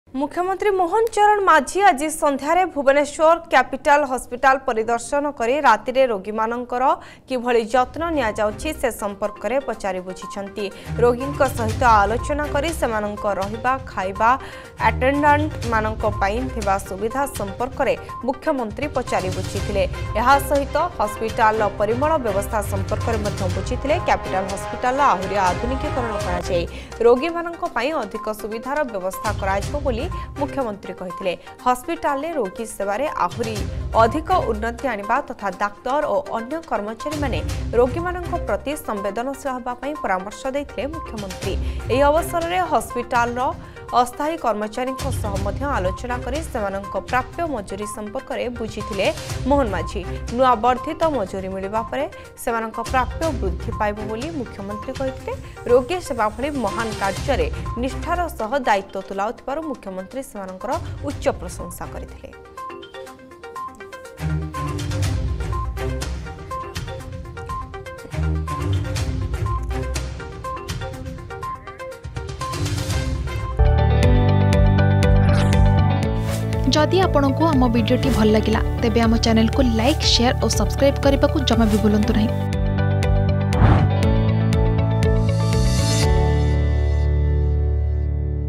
କ୍ୟାପିଟାଲ ହସ୍ପିଟାଲ ପରିଦର୍ଶନ କଲେ ମୁଖ୍ୟମନ୍ତ୍ରୀ || Mohan Charan Majhi || Capital Hospital
କ୍ୟାପିଟାଲ ହସ୍ପିଟାଲ ପରିଦର୍ଶନ କଲେ ମୁଖ୍ୟମନ୍ତ୍ରୀ । ରୋଗୀଙ୍କ ଯତ୍ନ କିପରି ନିଆଯାଉଛି ପଚାରି ବୁଝିଲେ ମୋହନ ମାଝୀ।